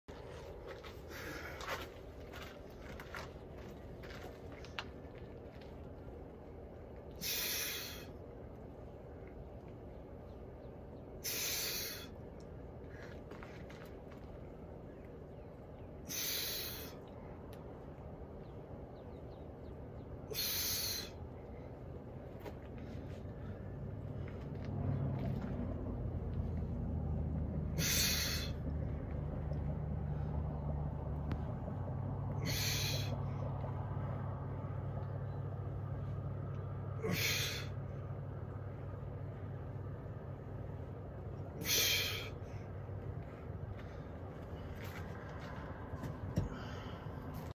DO NOT TRY THIS!! - Swinging Thor's Hammer for Gainz
8/3/21 - Swinging Thor's hammer; ~35 lb. Don't try this at home!